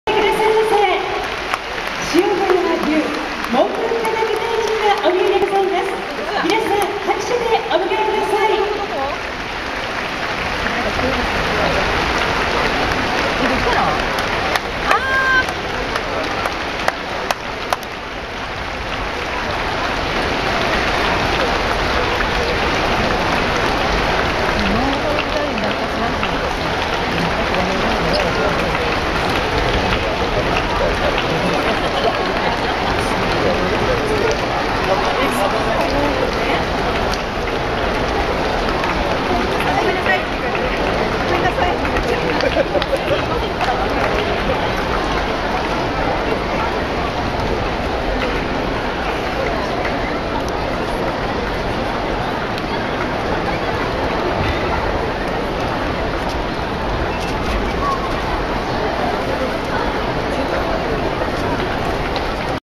塩谷文部科学大臣入場 Minister entrance
Minister of Education, Culture, Sports, Science and Technology entrance.
JKA boy and girls championship opening ceremony.
塩谷文部科学大臣がエコパアリーナに入場！